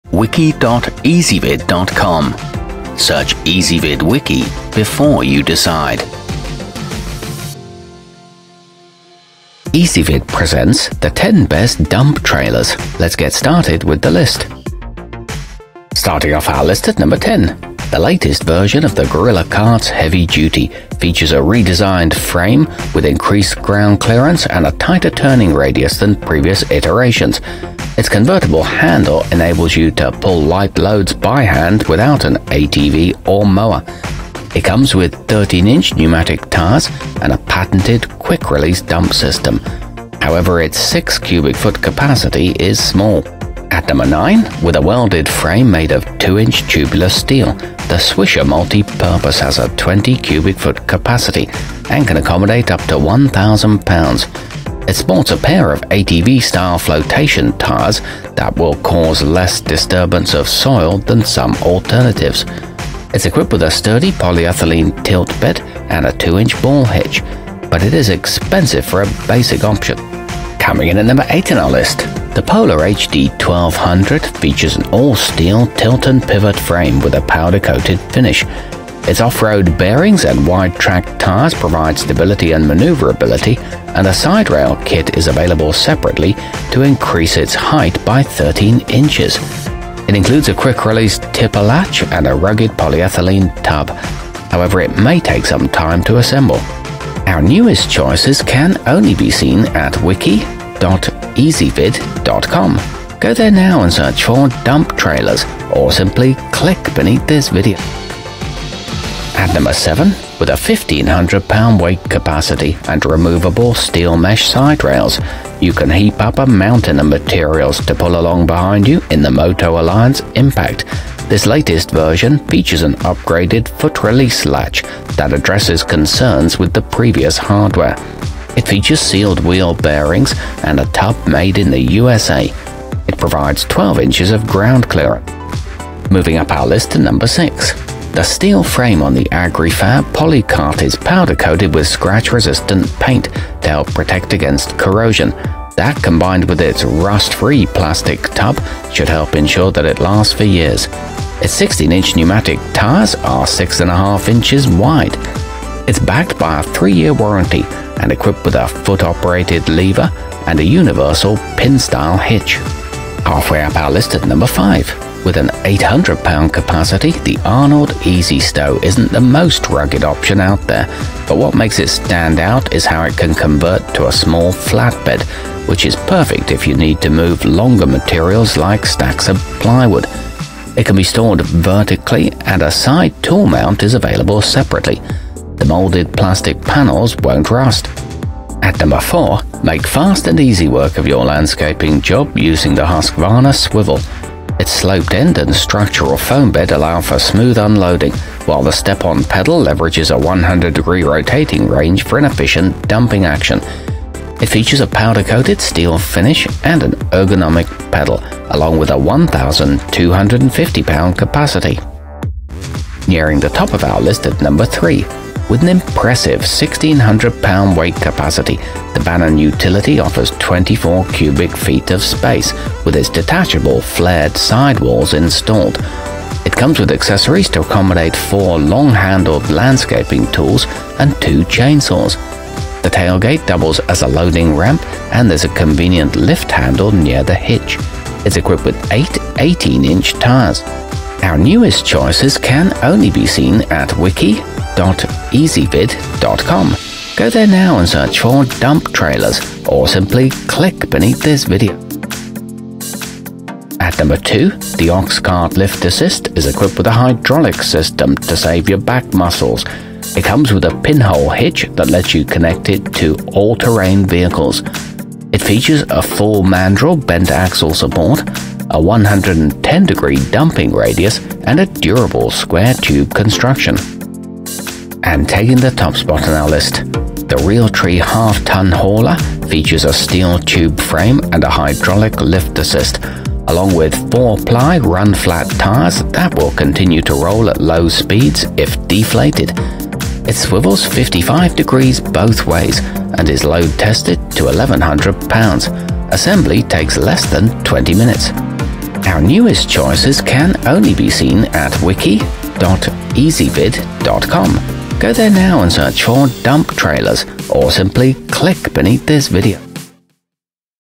10 Best Dump Trailers 2020
Our complete review, including our selection for the year's best dump trailer, is exclusively available on Ezvid Wiki.

Dump trailers included in this wiki include the bannon utility, realtree half-ton hauler, arnold ez stow, husqvarna swivel, oxcart lift-assist, agri-fab poly cart, swisher multi-purpose, motoalliance impact, gorilla carts heavy-duty, and polar hd 1200.

Dump trailers are also commonly known as tow behind dump carts.